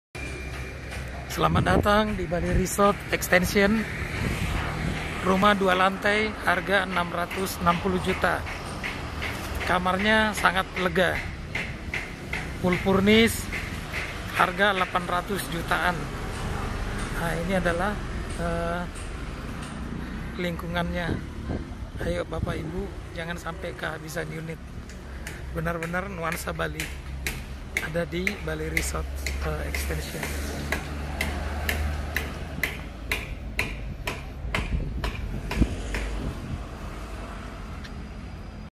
BALI RESORT EXTENSION SERPONGRumah Rp 660jt - 3KT, 3KM
Kunjungi Rumah Contoh
BALI RESORT EXTENSION SERPONG
Rumah Rp 660jt - 3KT, 3KM

Rumah mewah, dengan kamar tidur luas.
Bernuansa exotic Bali yg merupakan kebanggaan budaya Indonesia.

Lokasi dengan pusat kota BSD City, Teras Kota, Eka Hospital, AEON Mall, ICE BSD, The Breeze, Stasiun Intermoda Cisauk

Informasi :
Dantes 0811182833